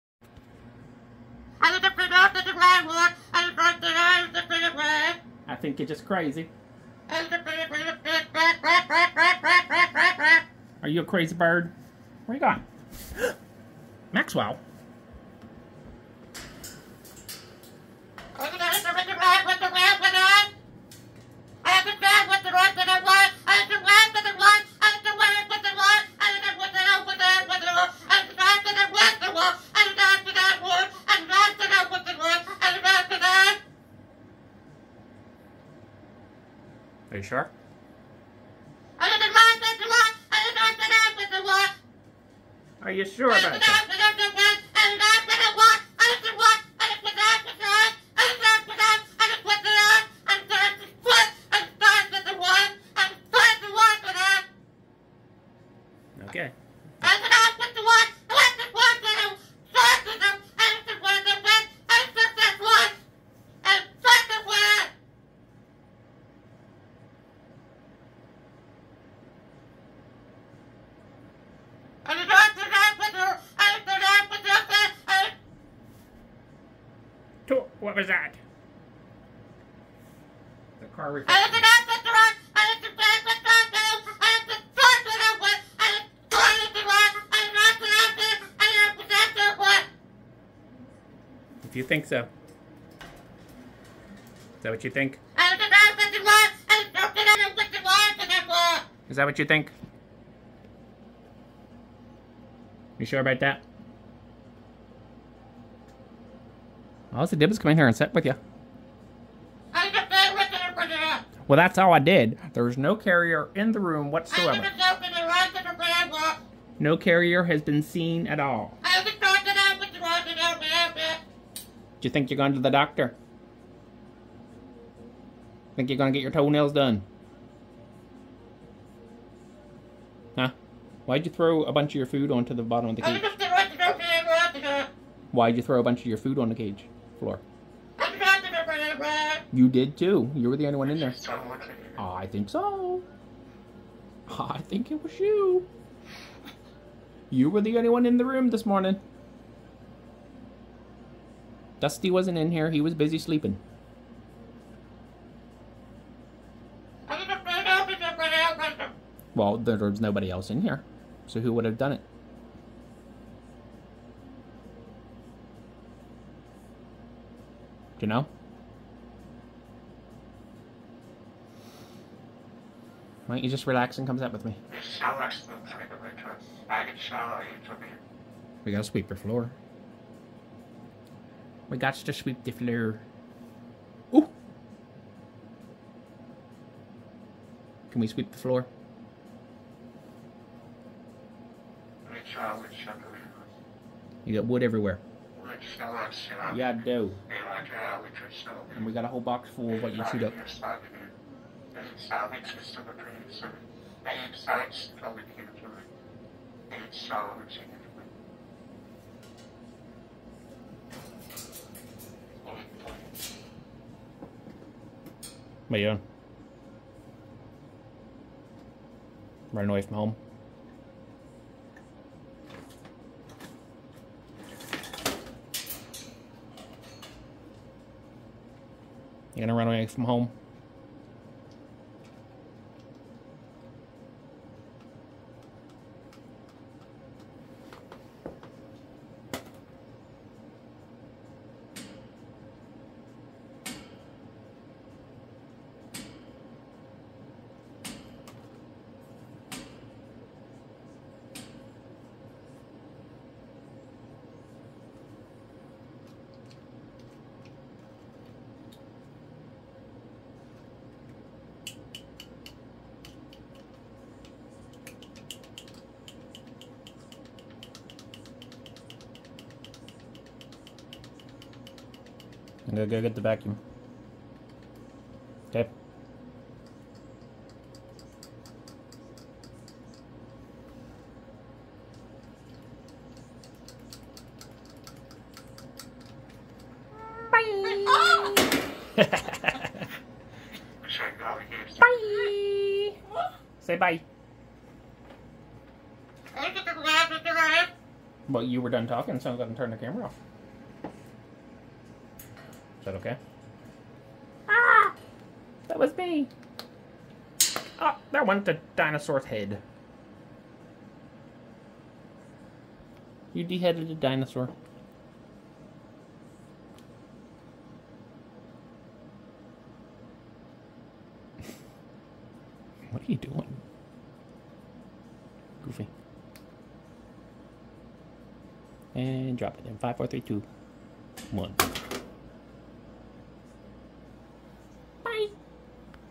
Cockatoo Prepares For A Vet Visit
Max didn't even see the carrier, but he knows what's coming.